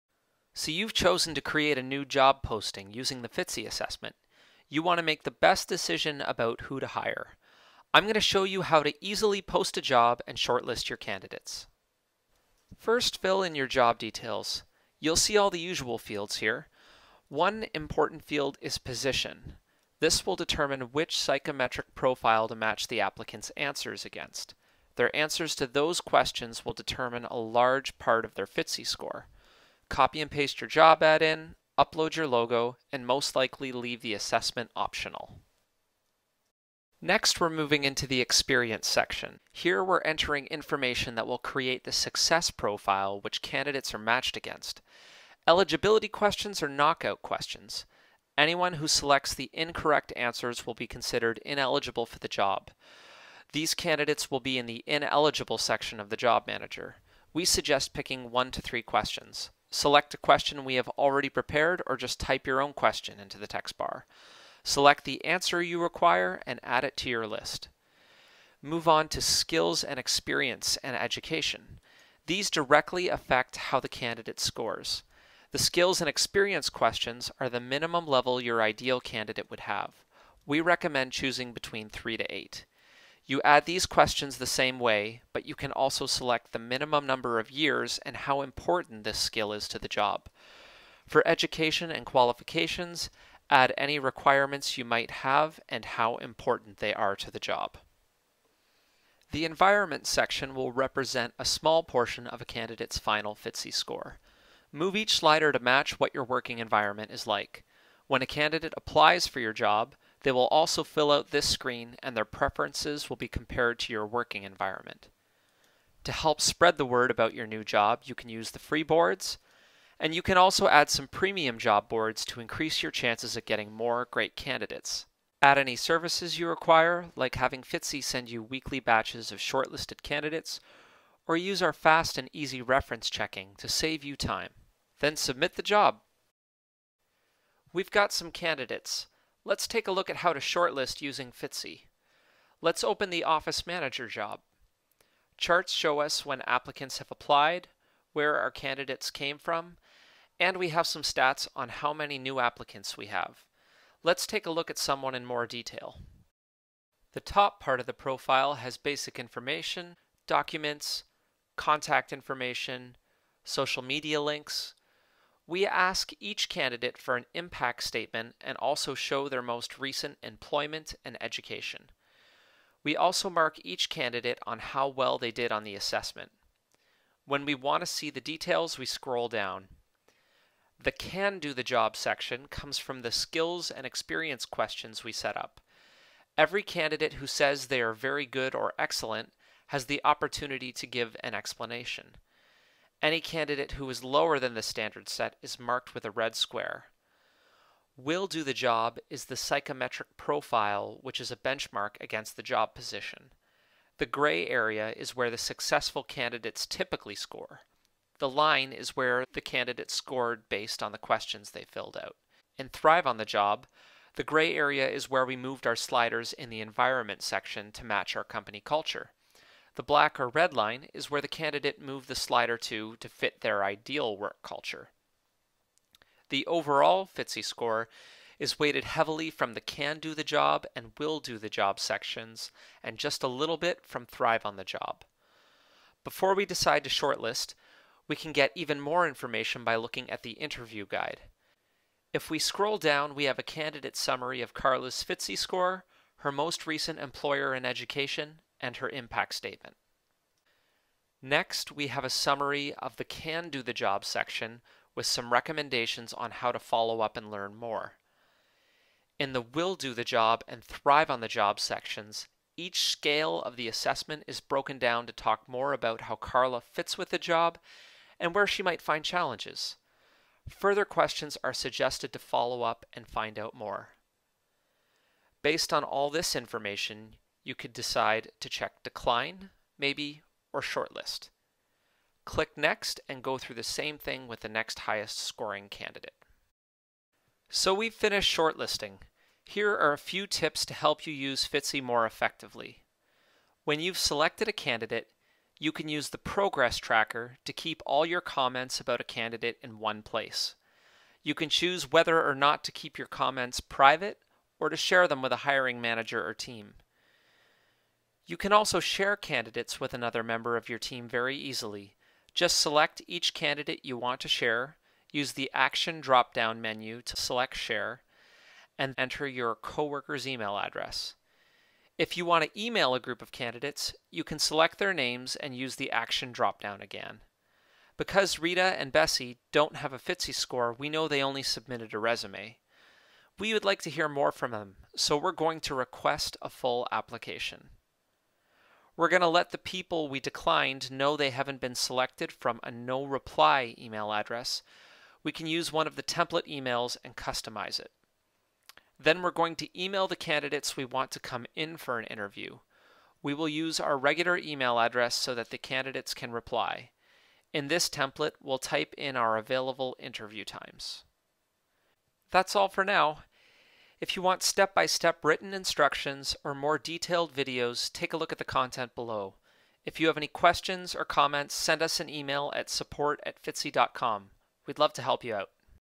Fitzii Overview: Post A Job, Shortlisting and Tips
1) Job Details:

o First fill in your job details. Most are easy to figure out. Here are some special ones to pay attention to:
o Job Title: Pick the most boring, common job title you can come up with. You want a title that job seekers will be able to find when they are searching job boards. So while you might call it an “efficiency elf” or “productivity prince or princess,” advertising it as an “executive assistant” or “office manager” will probably get you more applicants.
o Job ID: Optional, for internal use.
o Position: This will determine which psychometric profile to match the applicant's answers against. Their answers to those questions will determine a large part of their Fitzii score. Make sure this is as accurate as possible.
o Salary: You can choose to hide this by not clicking on the “Show Salary” button.
o Company Name: You can choose to hide the company name for a confidential job search by unselecting “Show company & logo.”
o ZIP/Postal Code: Should match the location of where the job will be.
o Business Unit: Optional, for internal use.
o Job Ad: Compose it directly in the software, or copy and paste from your Word program.
o Company Logo: If you have not selected “show company & logo” above you can upload a logo.
o Assessment: If you don’t know how many applicants you are going to get, we suggest leaving this optional. This way, if someone is in a rush they won’t pass over your job. You can always request that they fill out the assessment afterwards. If you do know you are going to have a lot of applicants, we would suggest making the assessment required. This helps limit your candidate pool to the people who are really interested in working for your company.

2) Experience:

o Here you’re entering information that will create the success profile which candidates are matched against.
o Eligibility questions are knockout questions. Anyone who selects the incorrect answers will be considered ineligible for the job. These candidates will be in the ineligible section of the job manager.
o We suggest picking one to three questions
o Select a question we have already prepared or just type your own question into the text bar. Select the answer you require, and add it to your list.
o Skills & Experience, and Education. These directly affect how the candidates score.
o Skills and Experience questions are the minimum level your ideal candidate would have. We recommend choosing between 3-8.
o You add these questions the same way, but you can also select the minimum number of years and how important this skill is to the job.
o Education & Qualifications
o Add any educational requirements along with the matching level of importance.

3) Environment:

o This represents a small portion of each candidate’s Fitzii score
o Move each slider to match what your working environment is like - each candidate will fill out the same form and a small part of their Fitzii score will be determined by how closely it matches your own setting.

4) Select Your Job Boards:

o Here you add job boards to help get exposure for your job ad.
o The Free Job Boards are SimplyHired and Glassdoor.
o Indeed is also free to use, but you need to unlock it. Click on the “unlock” button to receive instructions on how we can help you with this.
o The premium job boards (Workopolis, Monster, Linkedin and Careerbuilder) can be purchased for a great discount since we buy postings in bulk.

5) Services:

o Here you can add services to help make your hiring process more effective and efficient.
o Managed Shortlisting: One week after your job is posted, Fitzii will send you a list of the most qualified candidates who have applied. We will keep doing this for 4 weeks total. This is really helpful when you know you’re going to get a lot of applications, or if you’re really busy and should be spending your precious time elsewhere.
o Video Screening: Up to 30 shortlisted candidates can record video answers to five questions you select from our question bank. This allows you to see more people and get a fuller picture of who they are, resumes only show so much. You can review candidates whenever it’s convenient for you, and it allows you to be more objective since all candidates receive the same questions in the exact same tone of voice.
o Reference Surveys: Get quick and real feedback from a wider range of references. The surveys are filled out anonymously, so references can say what they really think about your candidate. The price listed covers all of the references for your job, you are not charged individually for each reference who fills it out.

6) Submit:

o Enter your credit card information.
o Then click on “Pay & Submit”